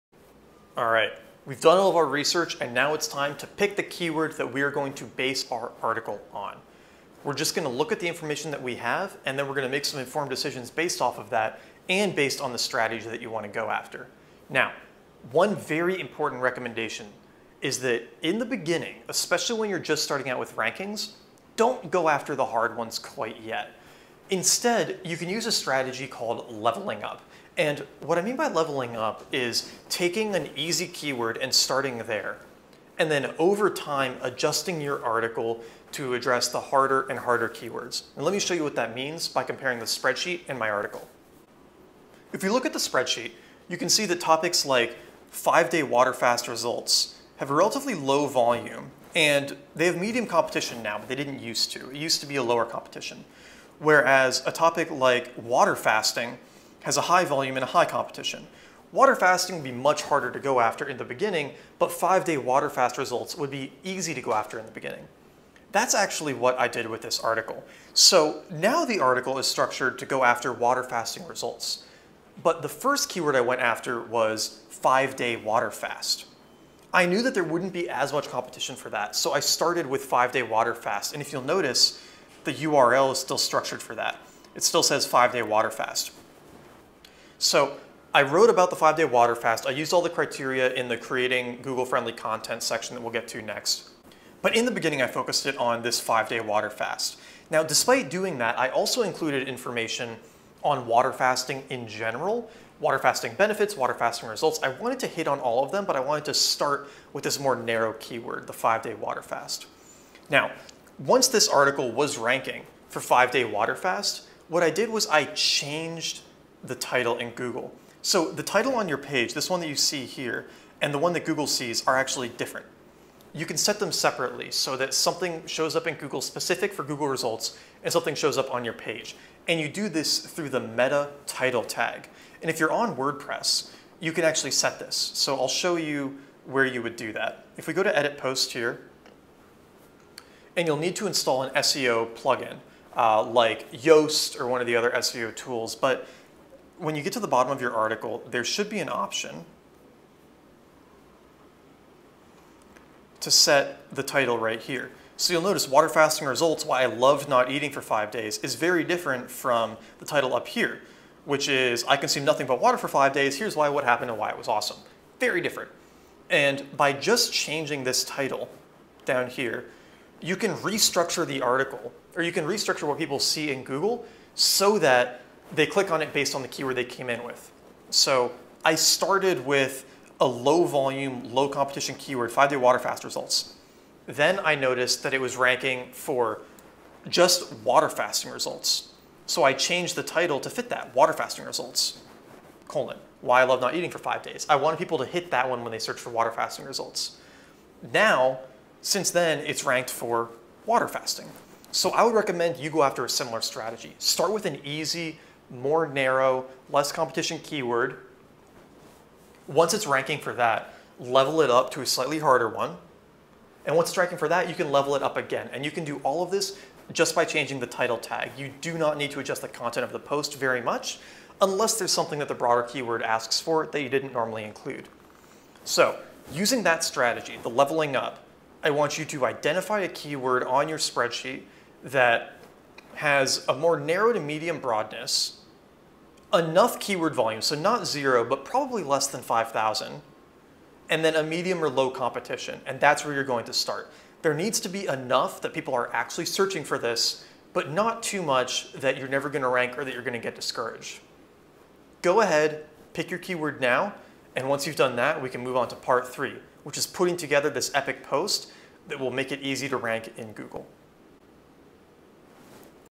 How to Find the Best SEO Keywords for Your Blog | 1-Hour SEO for Bloggers Pt. 13
1-Hour SEO for Bloggers is a comprehensive mini-course on how to land your content on the front page of Google in less than 1 hour per article.

In this video, learn how to select the best SEO keywords for your blog articles to maximize your traffic.